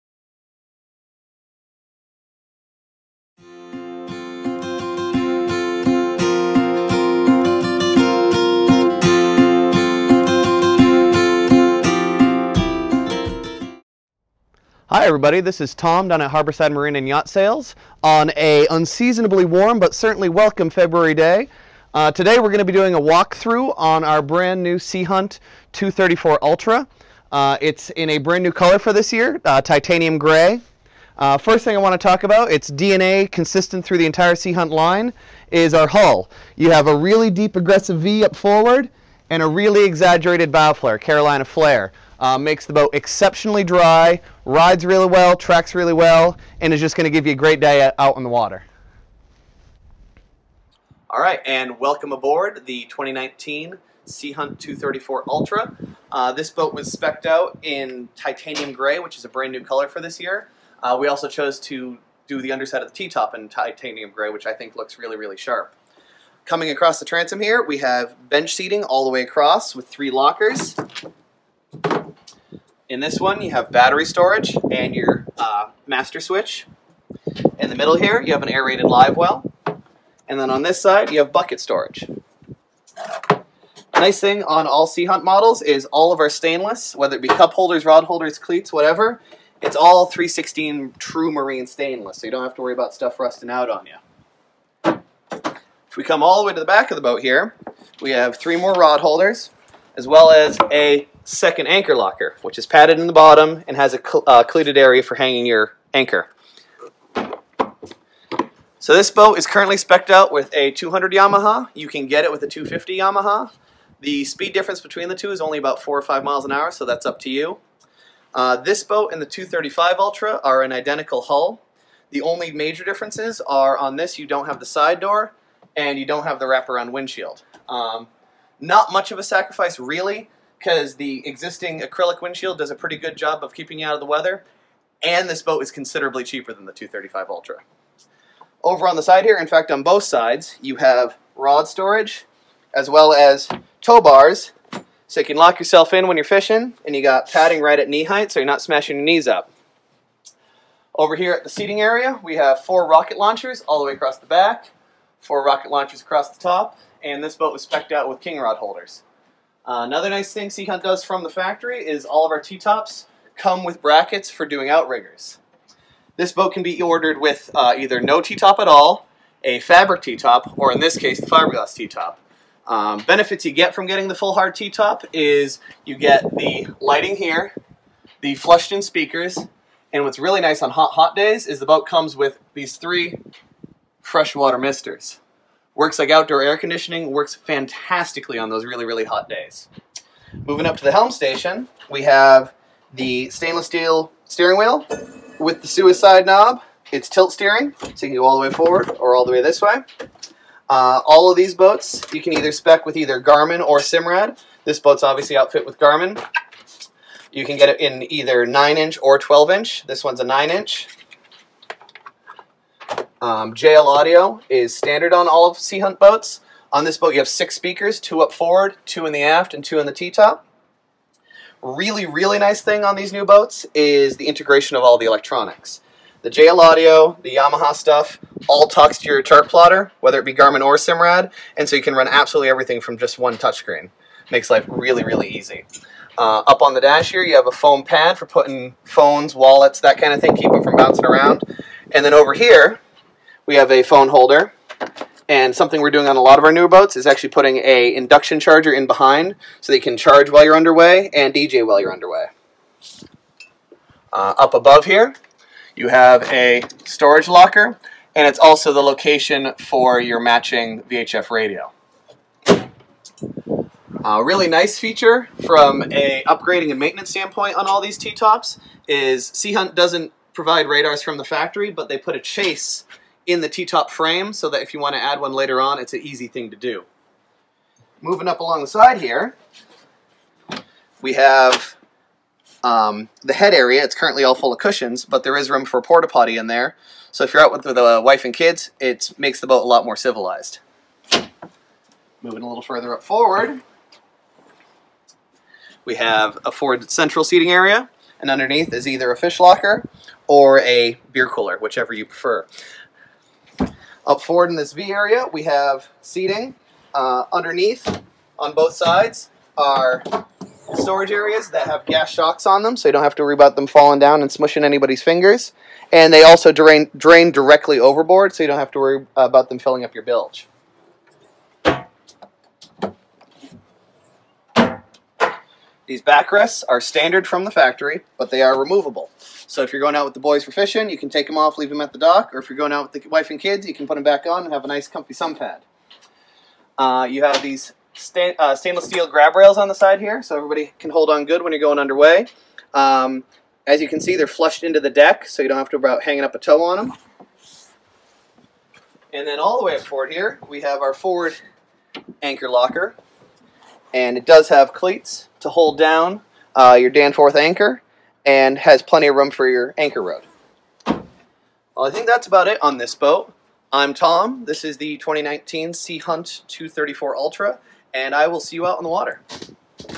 2019 Sea Hunt Ultra 234 Center Console For Sale at Harborside Marina
We took a walk around the new 2019 Sea Hunt Ultra 234   Watch to learn about all the great attributes this boat offers you.

This new boat is for sale at Harborside Marina just outside of Long Island, NY in Clinton, Conn on the New England Shoreline .
This boat is perfect for boating families looking to buy from a top boat dealer on the east coast focused on premium boaters, pontooners and fishermen.  It is equipped with a Yamaha engine and looks great.